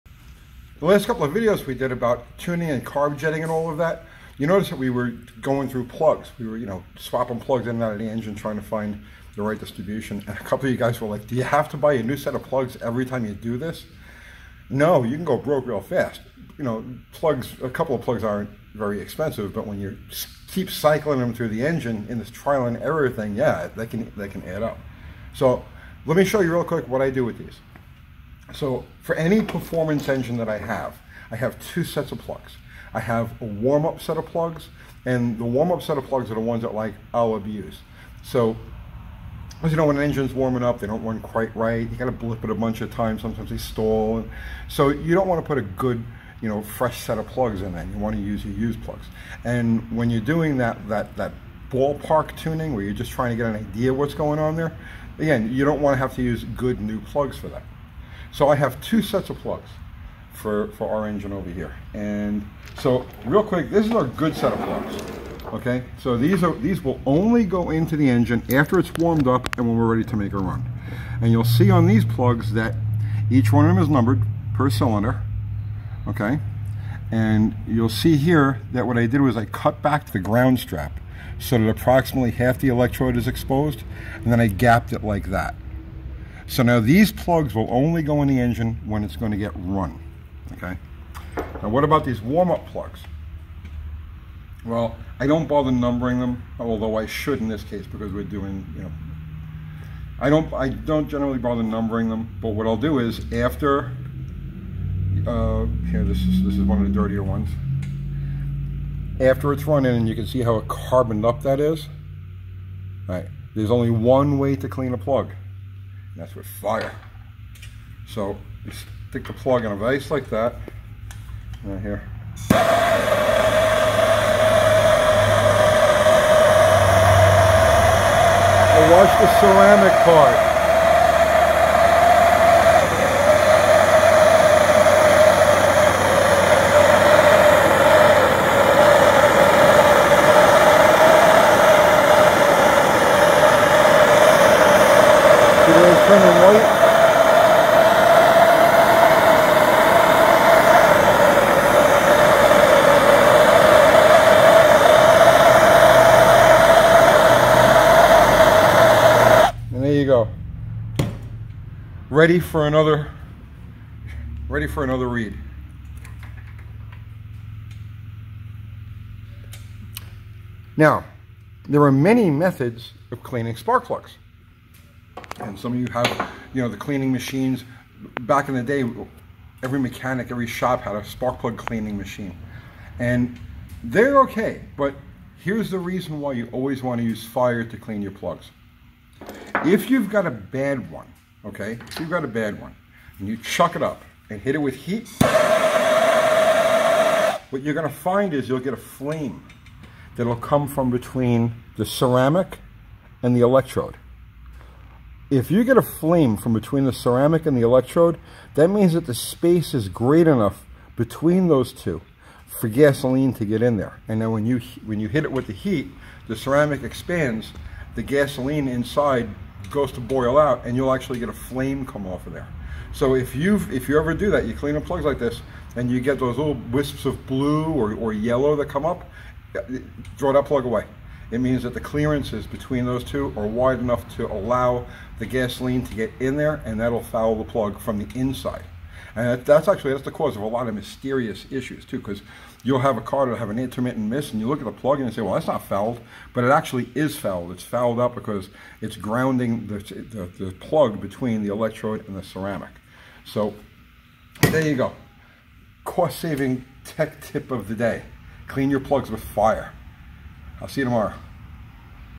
Clean It With FIRE!
Cycling through spark plugs in search of the perfect tune can be an expensive proposition, unless you know the secrets of getting them clean without using chemicals or abrasives.